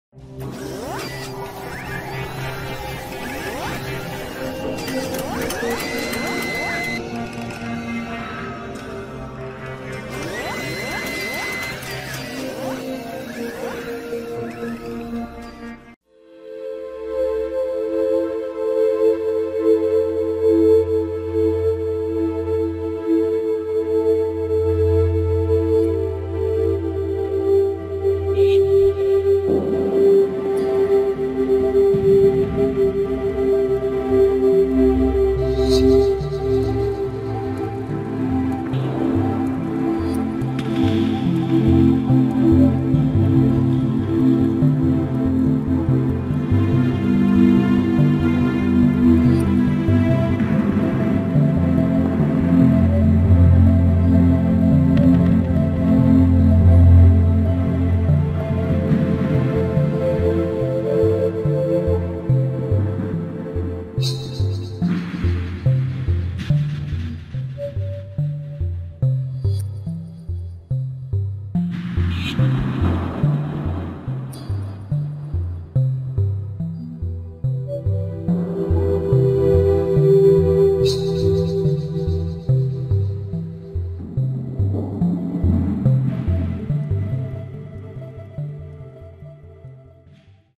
News. Fantastic - huge UFOs orbiting our sun. Pictures by NASA September 2, 2015
WOW 1:18. Look for updates on this channel. People should know the truth that hides NASA. Please help in the acquisition of the channel of the telescope for further research. NASA Hiding!